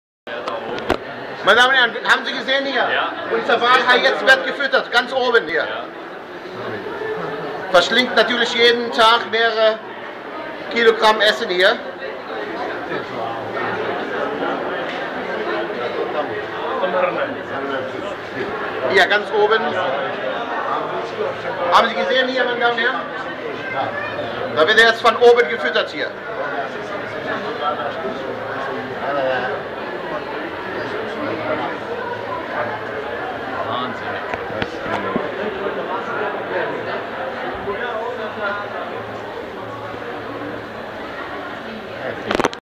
Feeding the Whale Shark "Sammy" at Atlantis Hotel Aquarium, Palm Jumeirah, Dubai. 15.01.2010.
Feeding the Whale Shark "Sammy" (Rhincodon typus) at Atlantis Hotel Aquarium, Palm Jumeirah, Dubai, United Arab Emirates. Video by: Dr.Sc. Norman Ali Bassam Khalaf-von Jaffa. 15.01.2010.
تغذية القرش الحوت "سامي" في أكواريوم فندق أتلانتيس ، نخلة جميرا في دُبي ، دولة الإمارات العربية المُتحدة ، يوم 15 يناير 2010 . تصوير فيديو : د. نورمان علي بسام علي طاهر خلف اليافاوي